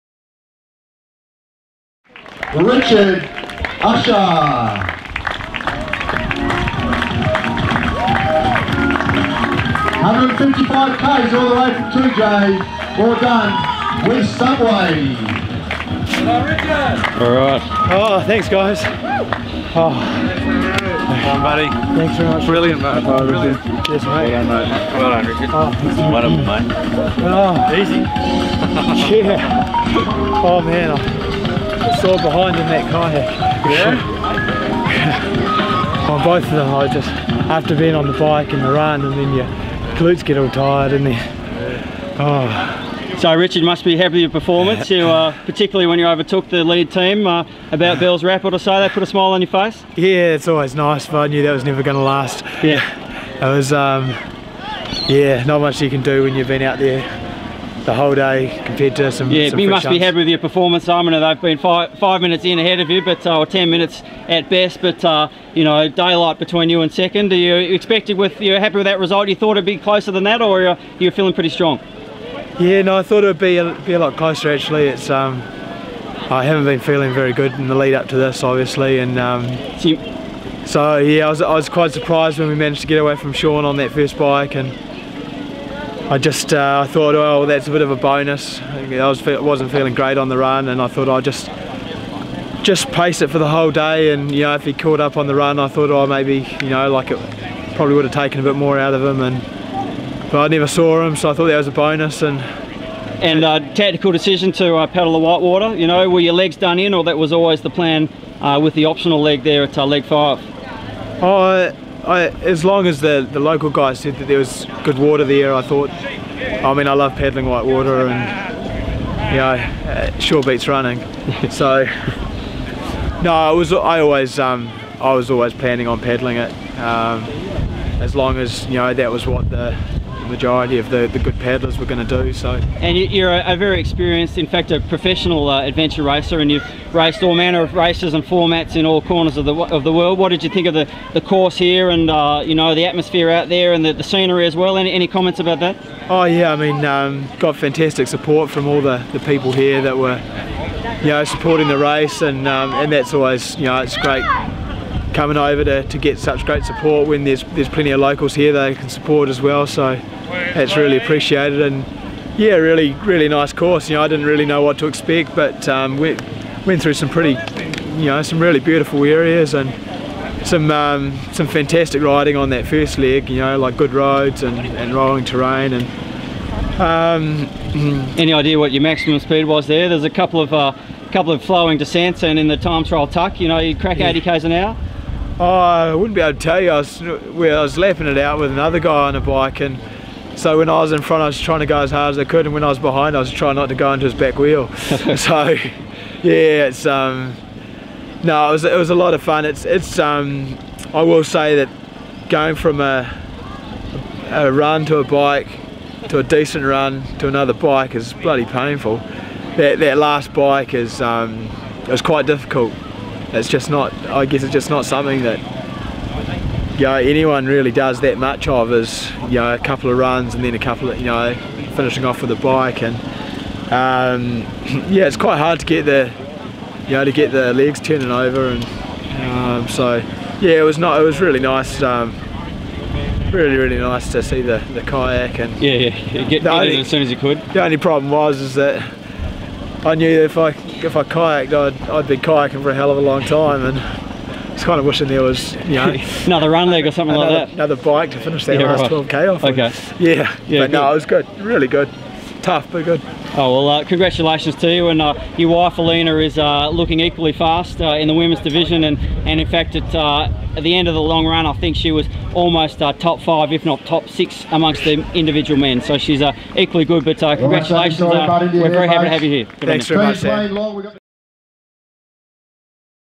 Richard Ussher Finish Interview.mpg
Richard Ussher has become the winner of the inaugural Mainpeak Multisport endurance event in Perth, Western Australia. Trail running, road riding and paddling his way through the Avon and Swan Valleys, he overcame man flu (which all us blokes knows is bloody serious) and an early charge from Perth local triathlete Sean O'Neill to sweep through and take the title in just over 8 hours. Here he chats just after crossing the line...